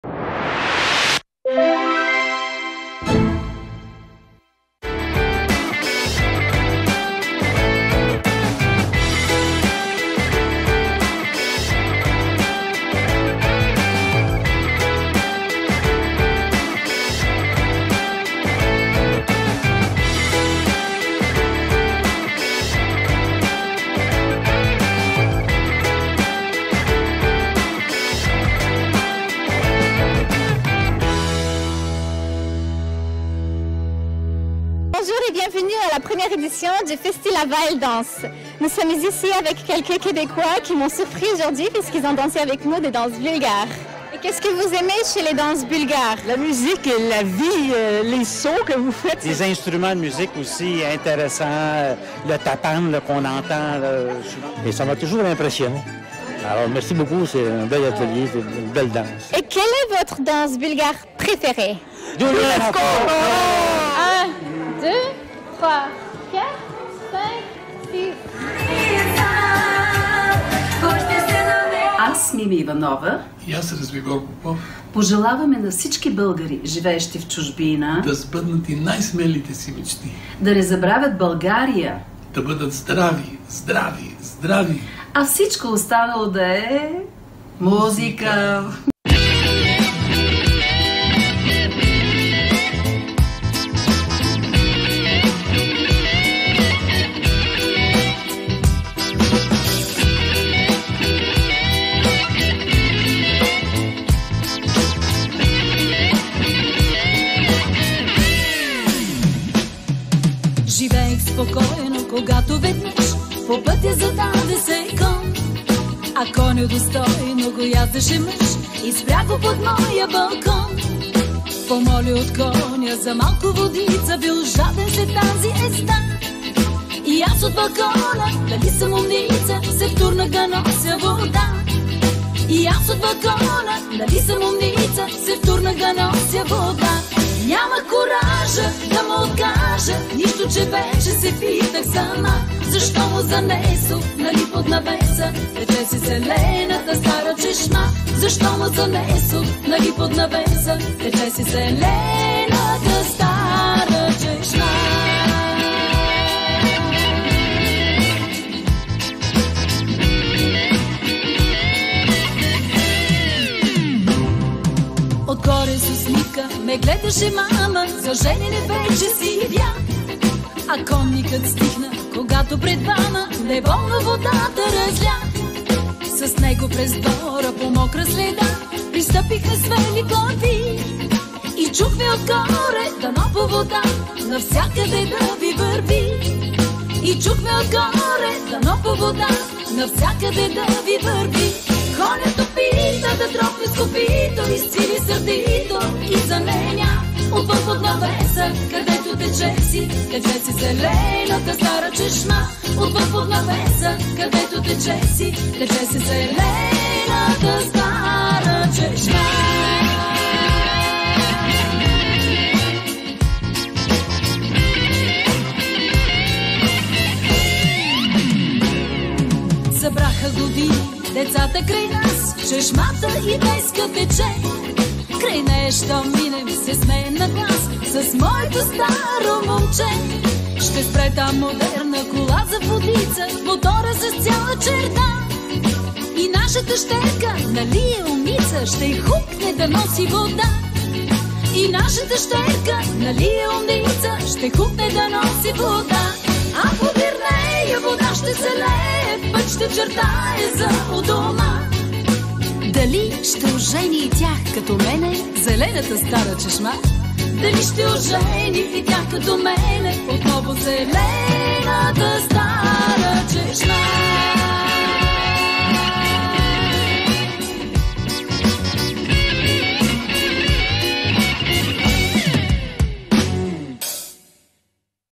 Local Quebecers Dance BULGARIAN & Mimi Ivanova ft. Razvigor Popov - Zornitsa TV
This special moment at FestiLaval Danse had thrilled us all! 😱 😍

This summer during the Bulgarian dance workshop lead by Bulgarian Folk Dance Ensemble Zornitsa, local quebecers fascinated us by their evident folkloric dancing talent !!

Watch a brief preview of our interview and greetings from two of our top Bulgarian performers, Mimi Ivanova & Razvigor Popov!

We are already looking forward to next year's festival for more fun on the dance floor! 🙌

Facebook.com/ZornitsaTV